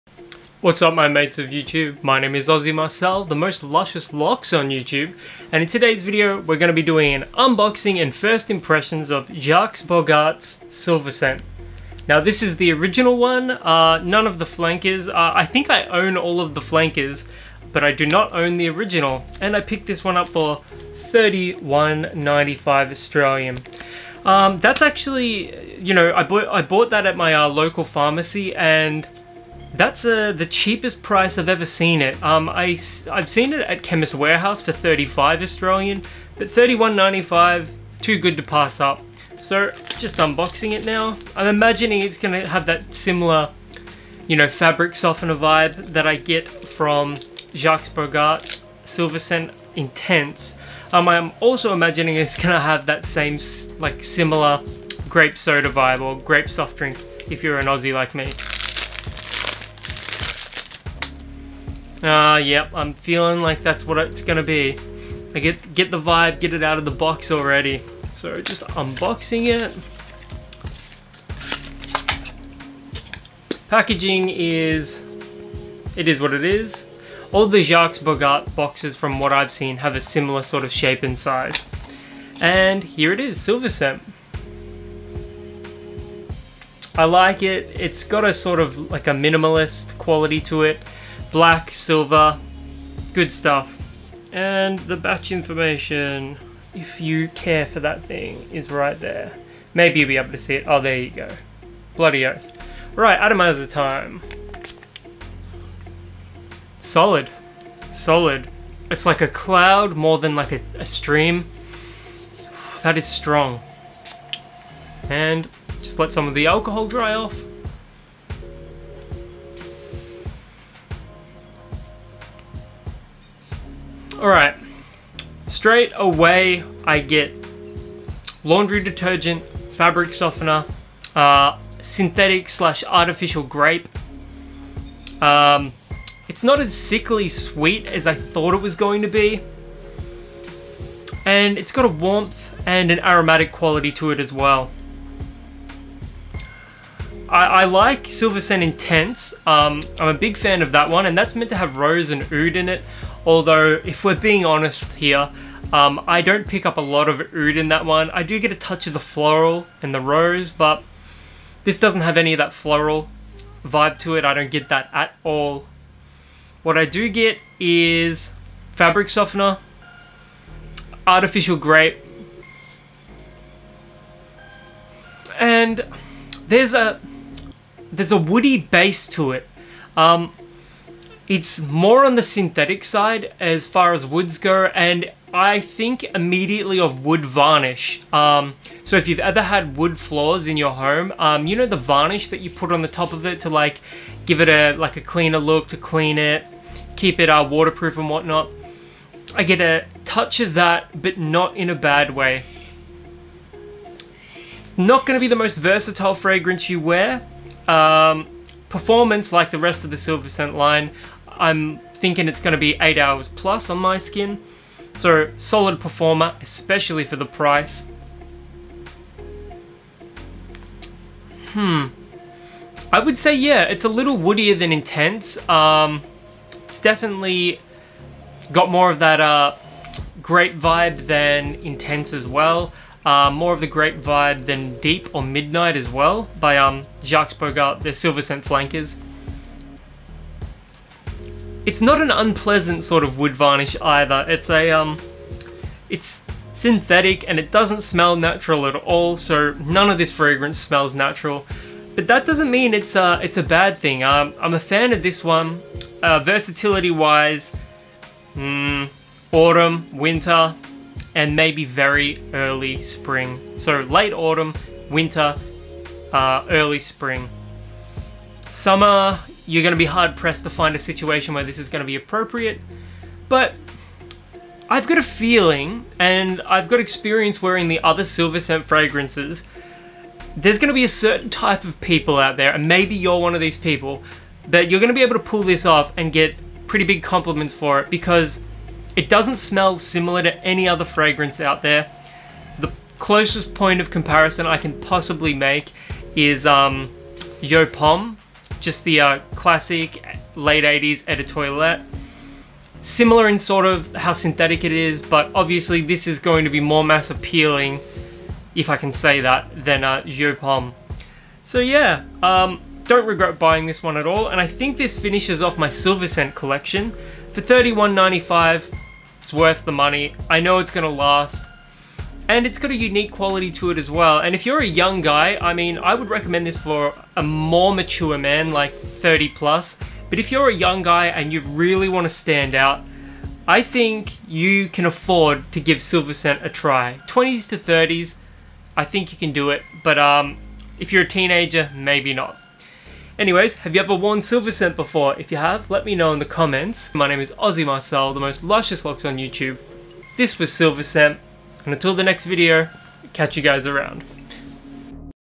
Jacques Bogart Silver Scent Men's Fragrance Unboxing & First Impressions
Today I'll be reviewing another men's fragrance, one that some would call a classic, Jacques Bogart Silver Scent. Enjoy!